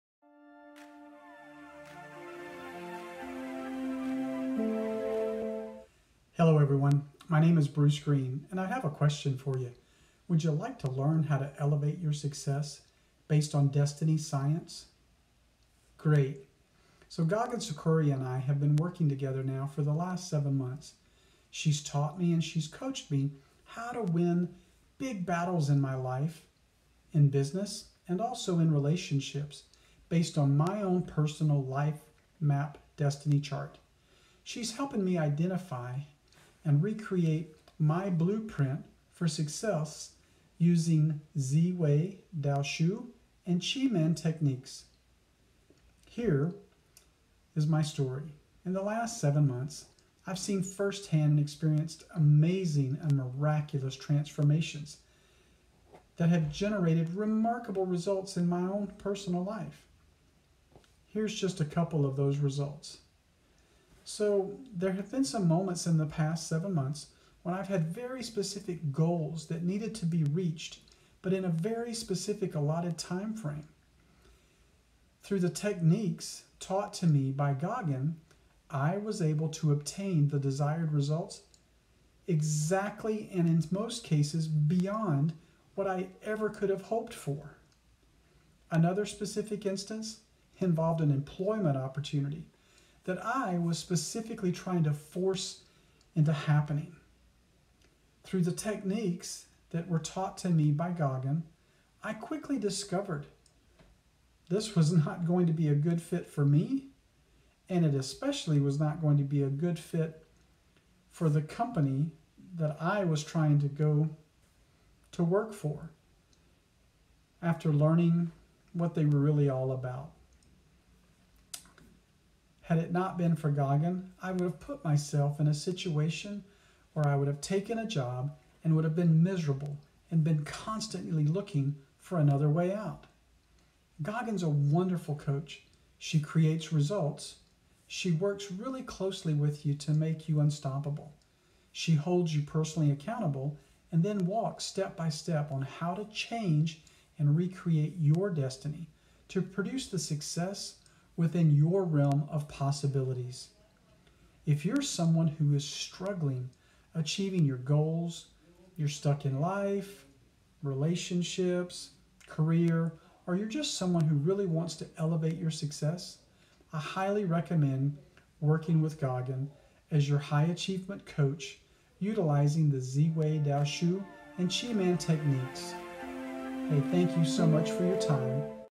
Gagan Sarkaria Client Bruce Green Mentoring Case Study
Soulful mentor and high achievement life coach Gagan Sarkaria worked with Bruce Green. Here he shares a case study of his experience.
Would you like to learn how to elevate your success based on destiny science?. Gagan and I have been working together now for the last seven months. She's taught me and she's coached me on how to win big battles in my life, in business and also in relationships based on my own personal life map destiny chart.
She's helping me identify and recreate my blueprint for success. Using Zi Wei Dou Shu and Qi Men Dun Jia Destiny Charts.
In the last seven months, I've seen firsthand experienced amazing and miraculous transformations that have generated remarkable results in my own personal life.
Here's just a couple of those results.
There have been some moments in the past seven months, when I've had very specific goals that needed to be reached. But in a very specific timeframe. Through the techniques taught to me by Gagan, I was able to obtain the desired results exactly, and in most cases beyond, what I ever could have hoped for.
Another specific instance involved in employment opportunity that I was trying to force into happening. Through the techniques that were taught to me, I quickly discovered this was not going to be a good fit for me. And it especially was not going to be a good fit for the company that I was trying to go to work for.
Had it not been for Gagan, I would have taken a job and been miserable and been constantly looking for another way out.
To learn more about how to work with Gagan, start with the Life Map Reading program or book a chat to discuss your goals and needs